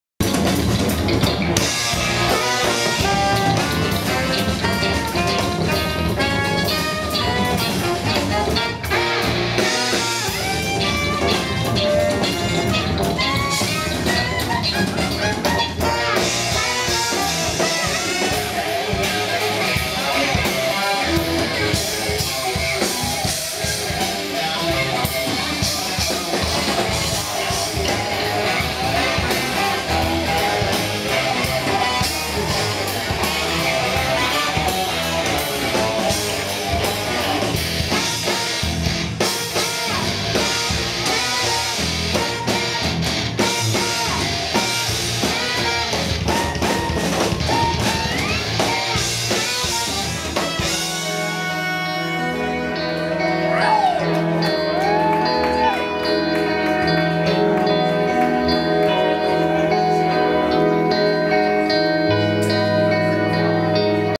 miRthkon at the New Parish, Oakland, CA July 2013
wally scharold - guitars, vox
 carolyn walter - clarinets, saxes
 jamison smeltz - saxes, vox
 matt guggemos - drums
 matt lebofsky - bass, vox
 travis andrews - guitar